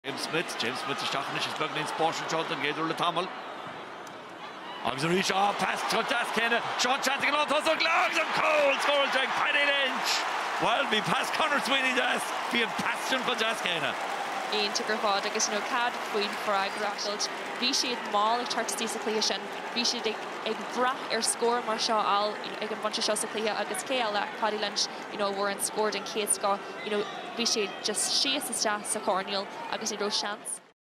Paddy Lynch Goal vs Tipperary - 2022 League Final - Cavan vs Tipp
Paddy Lynch’s 1st Goal of two vs Tipperary that earned him the AIB Man of the match. Division 4 league final 2022